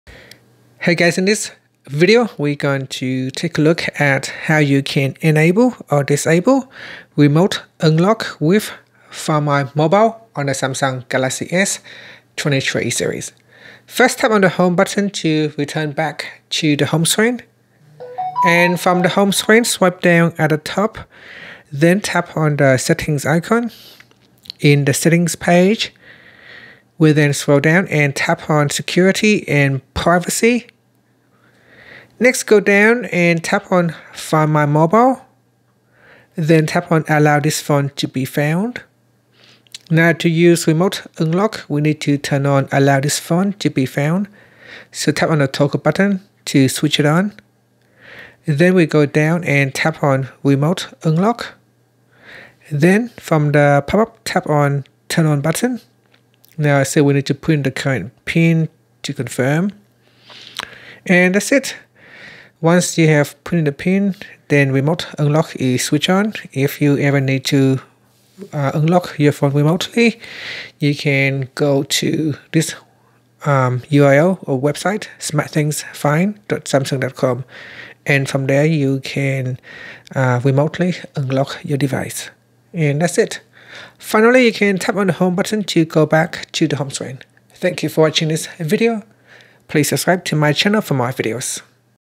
Galaxy S23's: How to Enable/Disable REMOTE UNLOCK With Find My Mobile
Learn how you can enable or disable REMOTE UNLOCK with Find My Mobile on the Samsung Galaxy S23/S23+/Ultra.

In this tutorial, we'll walk you through the step-by-step process of harnessing the power of Find My Mobile, a fantastic tool that enhances your smartphone's security and accessibility. Whether you're worried about losing your device or just want the convenience of unlocking it remotely, we've got you covered.

Benefits of Enabling Remote Unlock with Find My Mobile:

Enhanced Security: Protect your personal data and sensitive information by ensuring that only you can unlock your Samsung Galaxy S23, even if it's lost or stolen.

Lost Device Recovery: Find My Mobile lets you track your phone's location, so you can retrieve it quickly or take necessary actions to safeguard your data.

Convenience: Forgot your phone at home? No problem! Enable remote unlock to gain access to your device from anywhere, saving you from any inconvenience.

Emergency Situations: In case of an emergency, family members or trusted contacts can remotely unlock your device to access vital information or contact you.

Peace of Mind: Knowing that you have a failsafe way to unlock your phone in any situation provides peace of mind and reduces anxiety related to device loss.